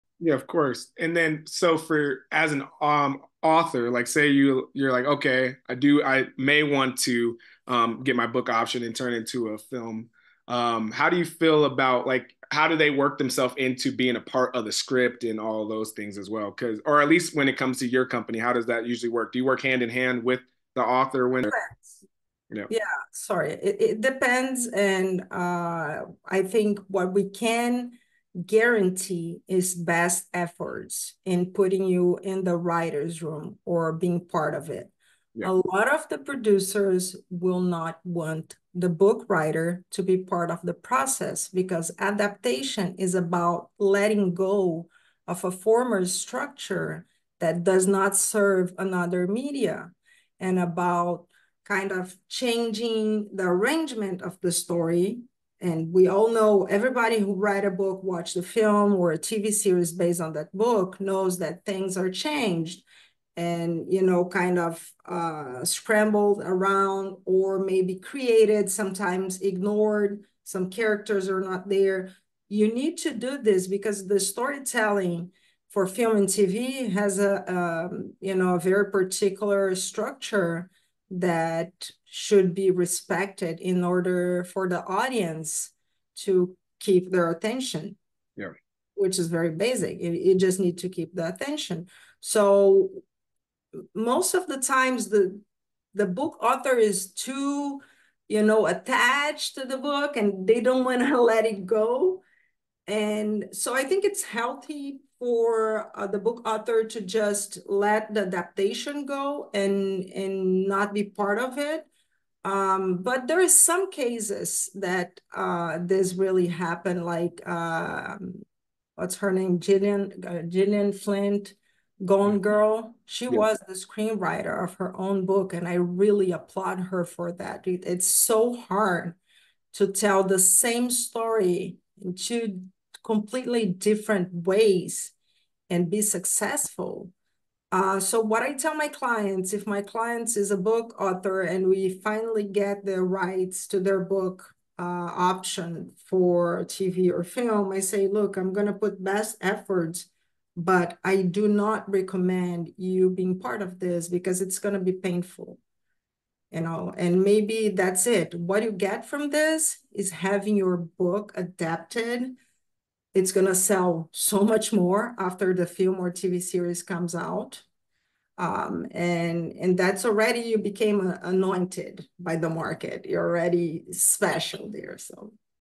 Should Authors Be Apart of the Movie/TV Adaptation Process?
Excerpt from the Bookum Nook Show and Founders Talk Podcast with Laura Malin.

Full Podcast Interview - @bookum